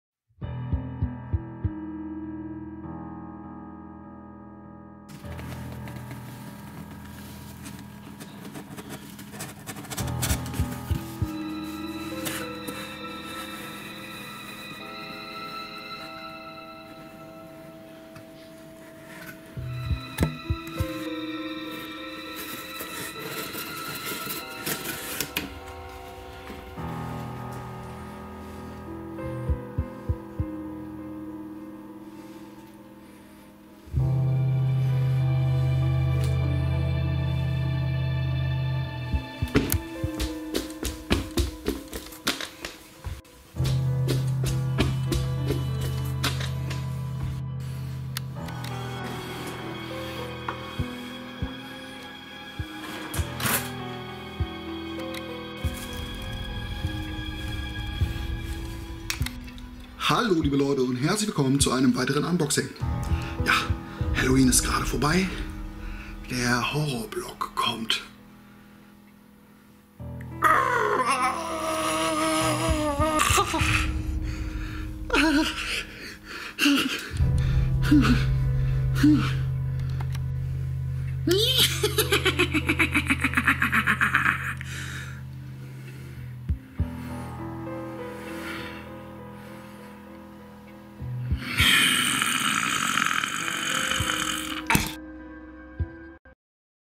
Silent Mike spielt mit Plüsch #002 - After-Halloween-Halloween-Horrorblock-Unboxing - Das Intro
Hier wieder eines meiner Intros als Einzelstück, ohne das laaaaaange Unboxing danach. Viel Spass!

Funko Sammelkiste - Die Facebook-Gruppe für alle Sammler!